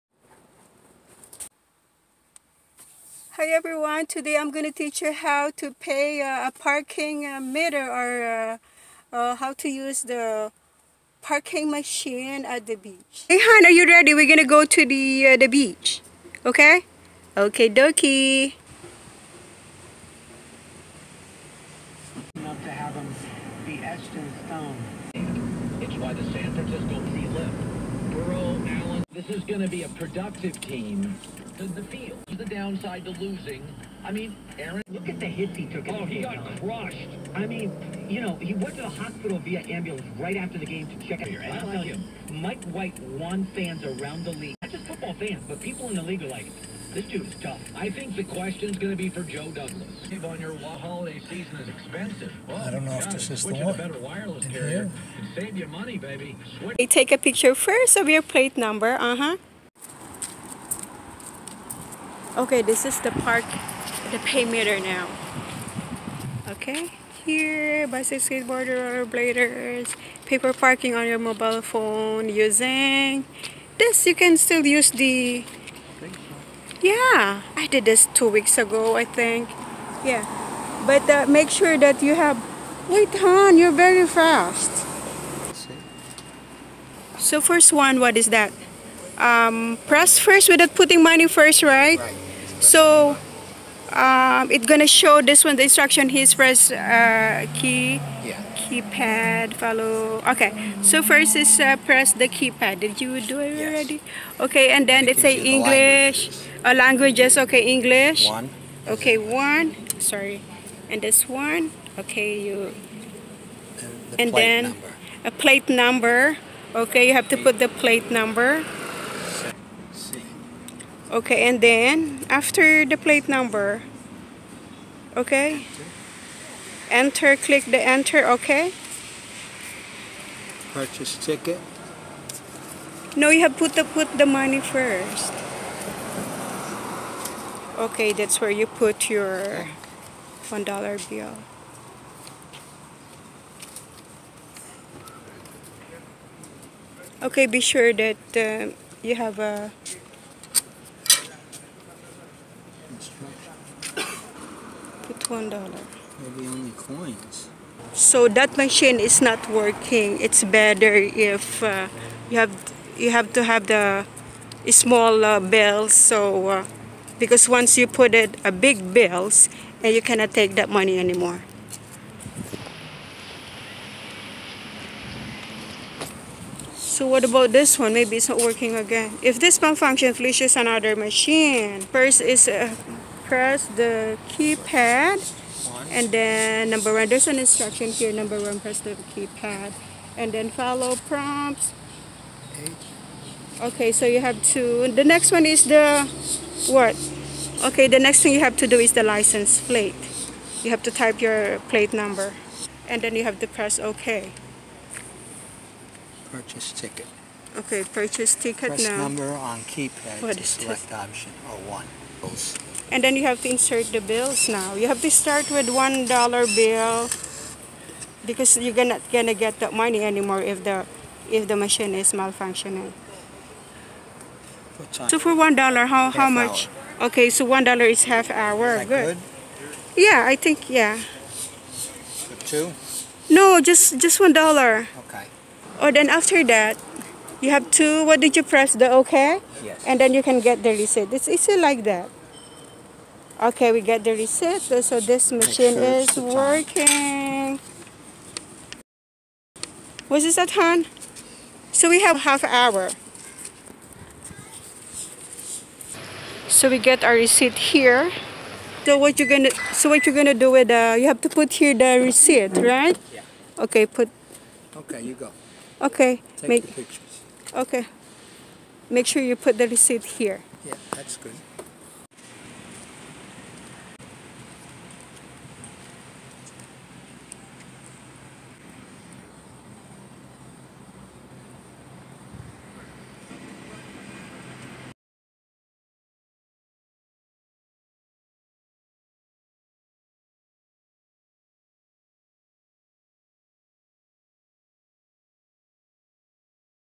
ON OUR WAY TO THE BEACH | HOW TO USE A PARKING METER AT THE BEACH || CITY OF POMPANO BEACH FL
City Of Pompano Beach | South Florida
It was late in the morning when we decided to stop by at the beach for a little while, so we can show you how to use the parking meter, and share the best tutorial we can. We passed at least  3 famous beaches in South Florida; Las Olas beach Fort Lauderdale, beach at Lauderdale-by-the-Sea, and last is Pompano Beach where I did my video.

So today we are using the old fashion way of paying the parking. There's more parking machines like this everywhere here in Florida, and although you can use pay for parking on your mobile phone using: Pay By Phone ( paybyphone.com ) or Passport Parking (passportparking.com ) in this type of machines as well, but this one for me is much easier too.

THE FIRST THING TO DO, IS TAKE A PICTURE OF YOUR PLATE NUMBER if you don't memorize it, so you don't have to go back again and again to your car. Thank you.